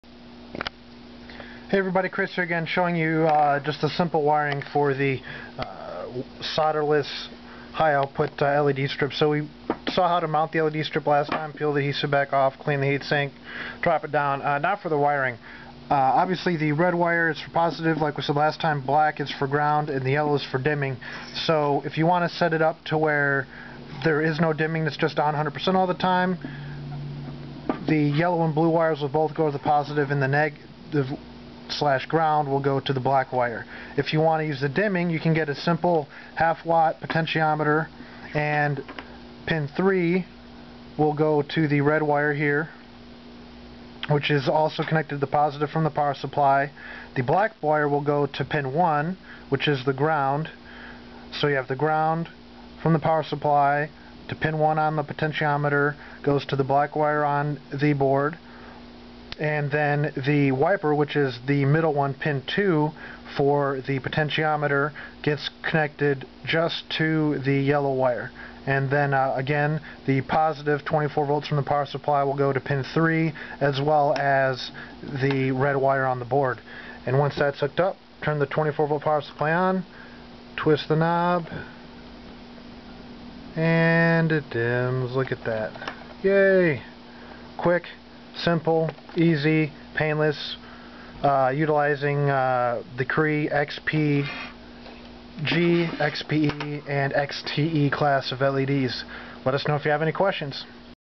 Solderless LED Kit vid. 2 wiring.AVI
The board comes complete with :

- Mounted LEDs (holds six LEDs - Cree X footprint; XP-G, XT-E, and XP-E - depending upon color spectrum requested)
- Integrated LED driver with dimming function  ( uses PWM - pulse width modulated dimming)
- Driving current of 700 or 920 mA
- Mounting adhesive
- Lead wires

This board runs off of a 24 vdc power supply (not included).  Multiple boards can dim together with one potentiometer, as long as they are run off of the same power supply. for a limited time get a free piece of aluminum extrusion with your order.